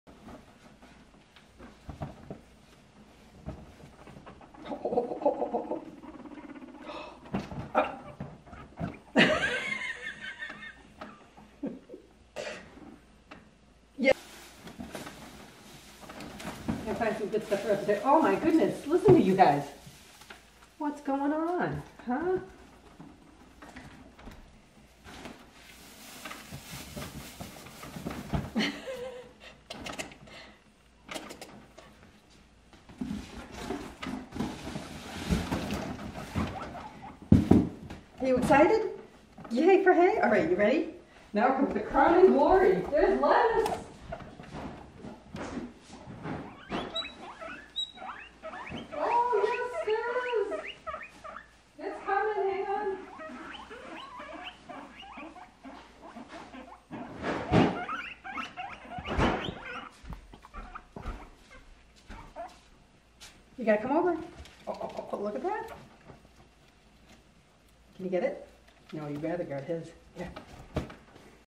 Bleu and Provolone say Yay for hay!
Dakin guinea pig fosters Bleu and Provolone are excited for their swept-out cage because they know hay and lettuce are coming soon!  As of this posting, they're available for adoption!